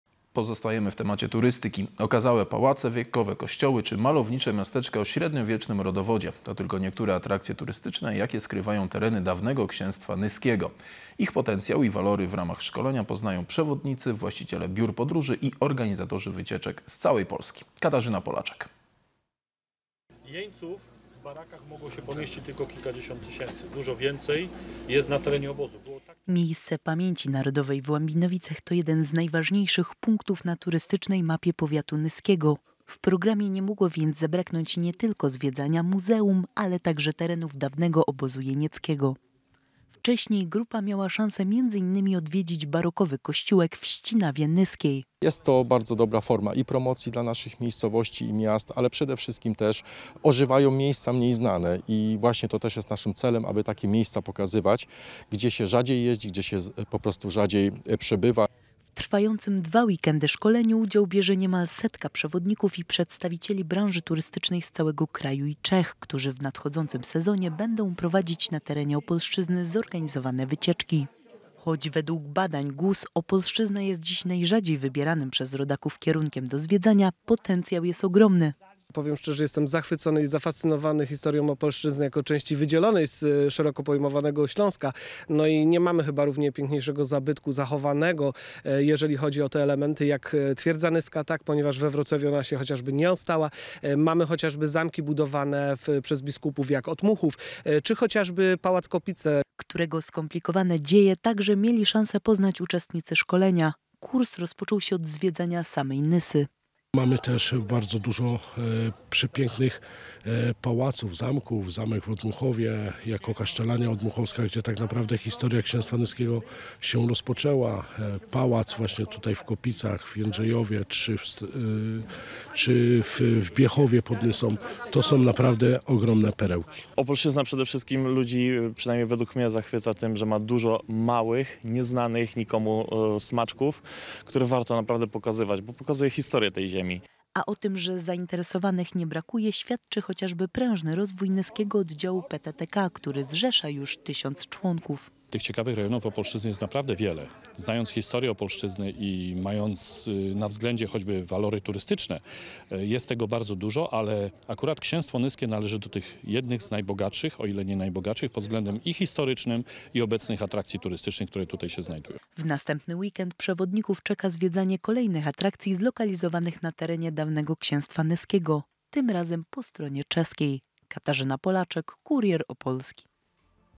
Szlakiem nyskich pereł. Przewodnicy turystyczni szykują się do sezonu
Okazałe pałace, wiekowe kościoły, czy malownicze miasteczka o średniowiecznym rodowodzie to tylko niektóre atrakcje turystyczne, jakie skrywają tereny dawnego Księstwa Nyskiego. Ich potencjał i walory w ramach szkolenia poznają przewodnicy, właściciele biur podróży i organizatorzy wycieczek z całej Polski.